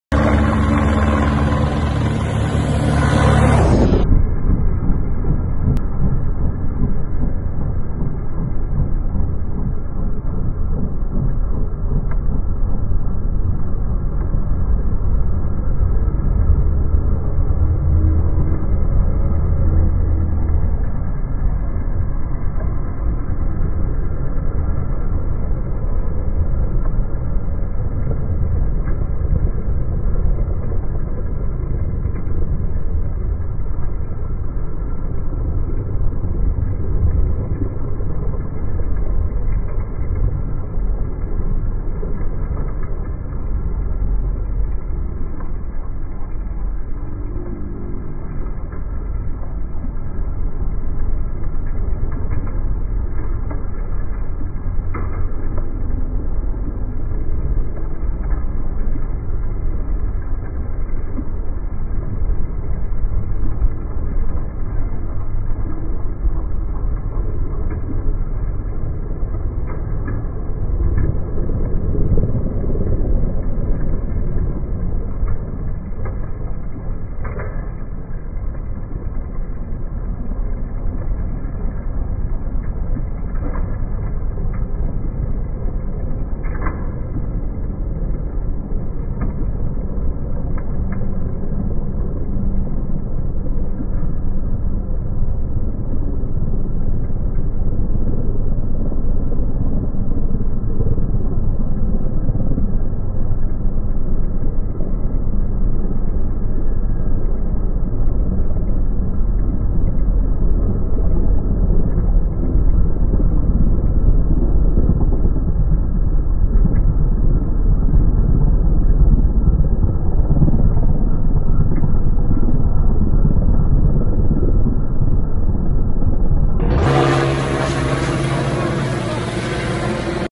Centrifugo, opera di Umberto Cavenago a Peccioli
La scultura di quattro metri per quattro fa bella mostra di sé in una delle ultime curve della 68esima Coppa Sabatini, corsa internazionale di ciclismo che si tiene a Peccioli, in provincia di Pisa.